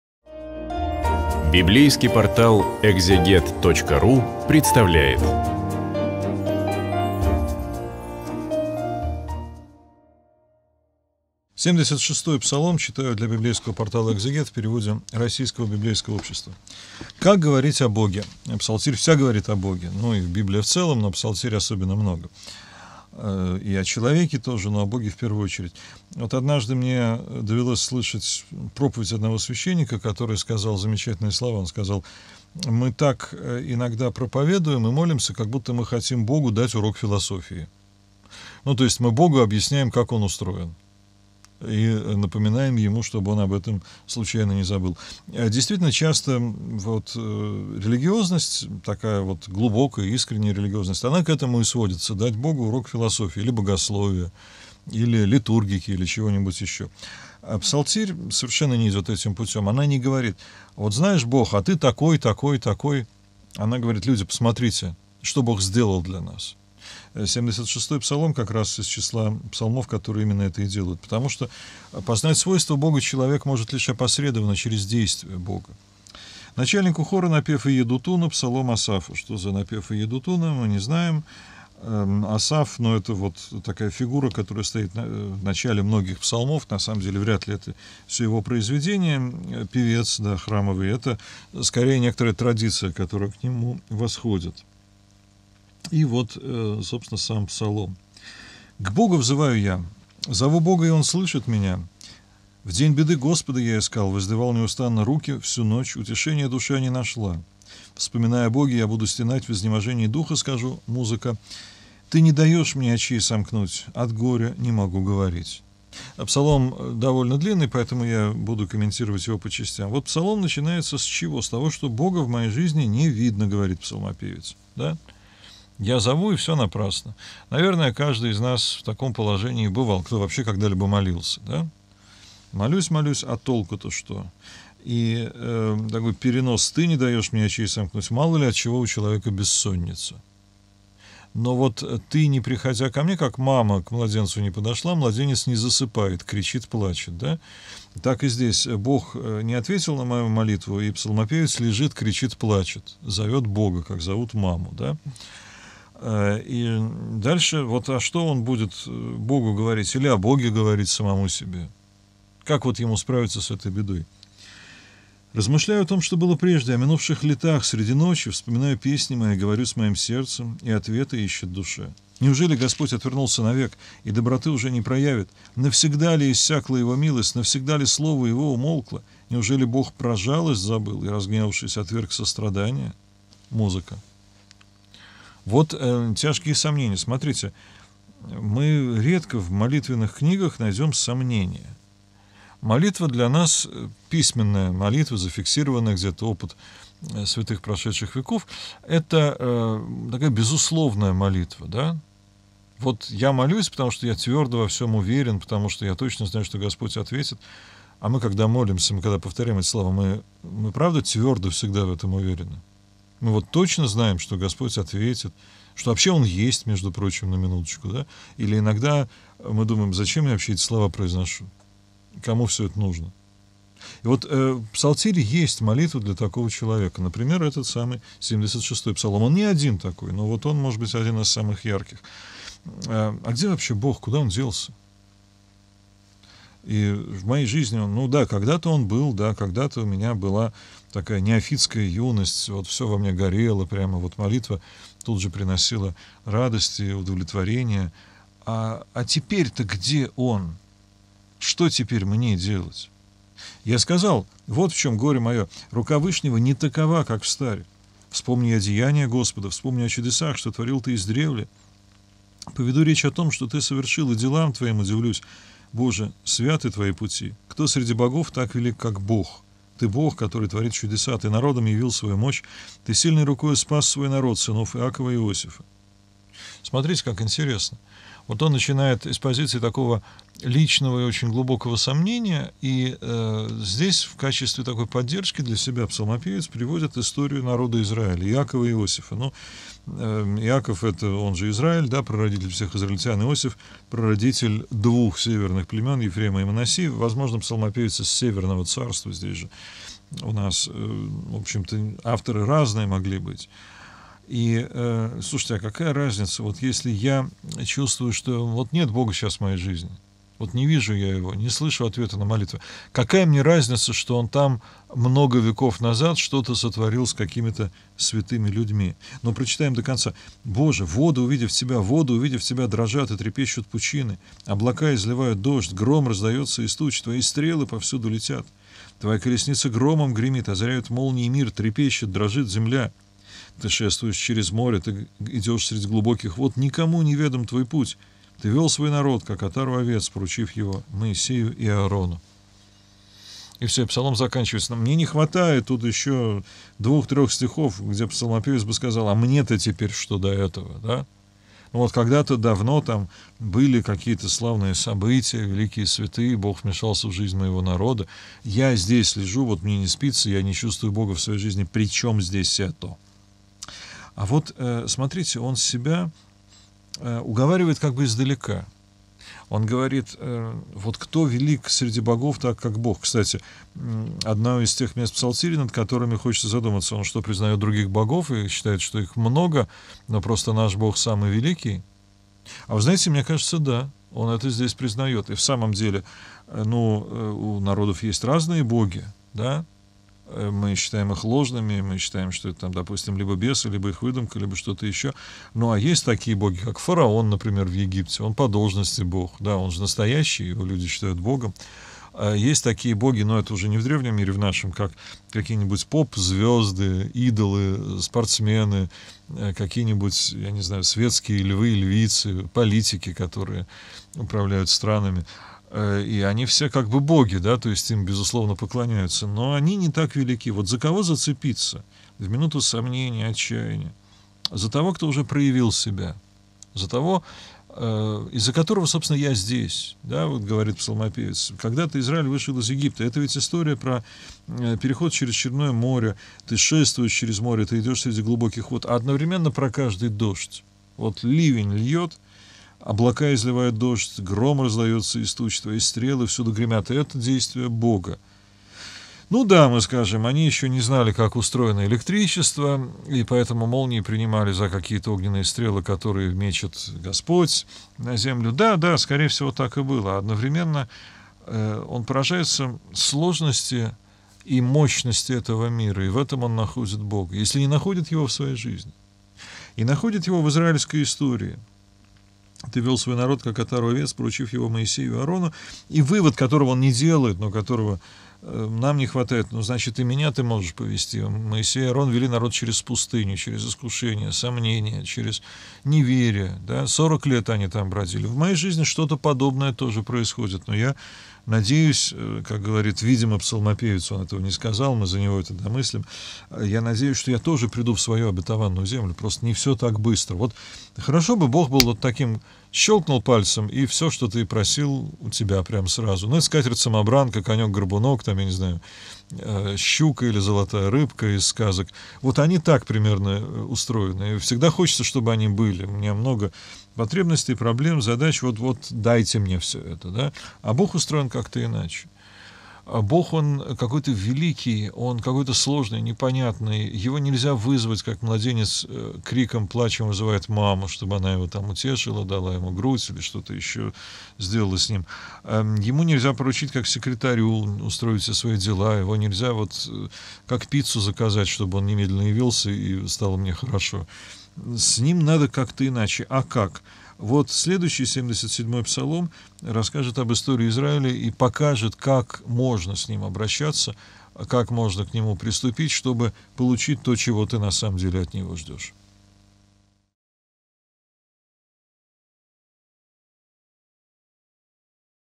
Псалтирь. Псалом 76. Андрей Десницкий. Библия
Продолжаем читать Псалтирь в переводе РБО, вместе с профессором Андреем Десницким. Разбираем 76-й псалом.
Псалмопевец взывает к Богу в молитве, говоря, что Бог его не слышит. Его обуревают тяжкие сомнения, ему кажется, что Господь его забыл.
Псалмопевец вспоминает чудеса, сотворенные Богом в прошлом с израильским народом и пытается укрепиться в мысли, что Бог всё-таки его не забыл и сотворит ему милость.